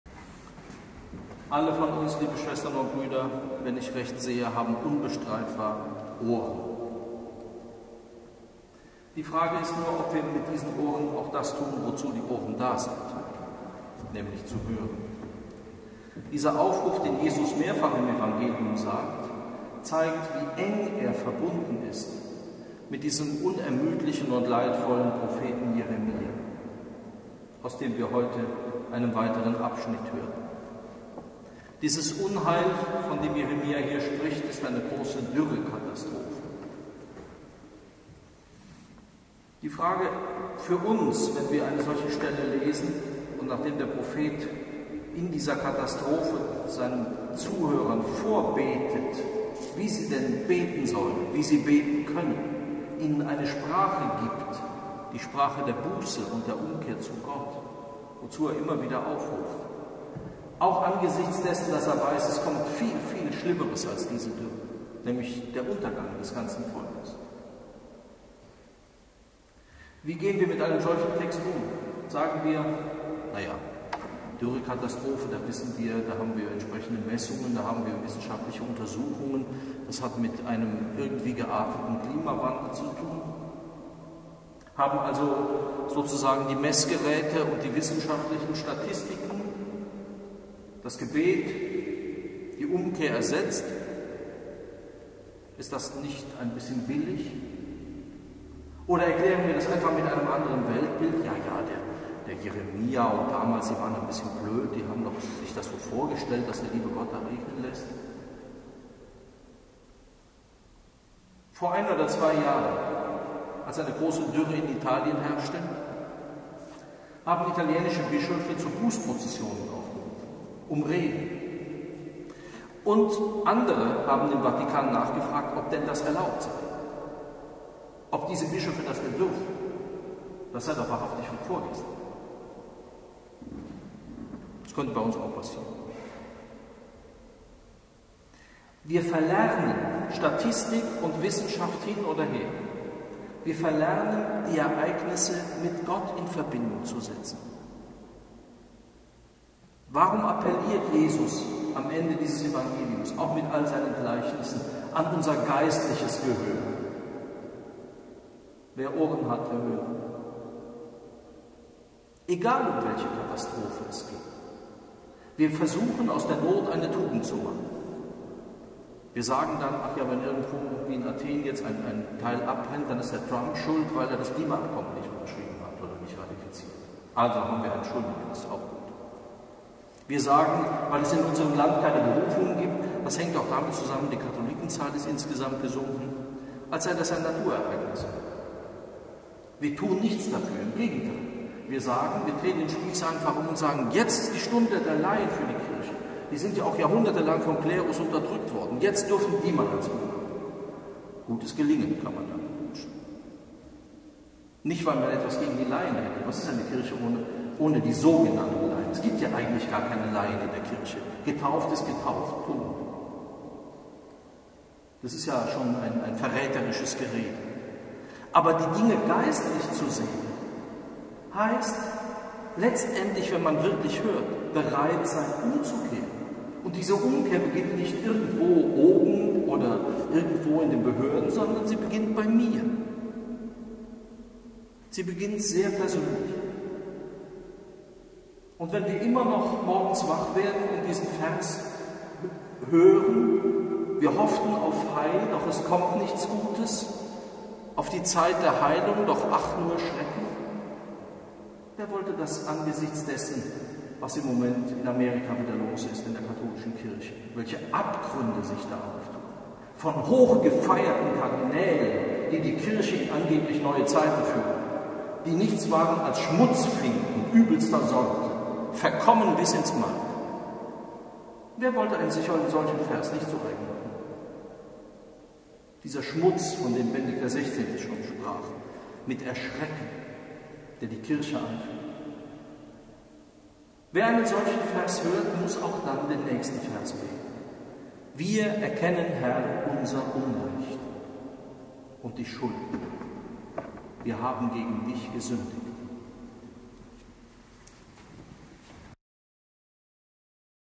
Was wir zunehmend verlieren
Preigt von Rektor Dr.Johannes J.Kreier am Di der 17.Wo im Jahreskreis im Kloster am Rande der Stadt/ SB 2018.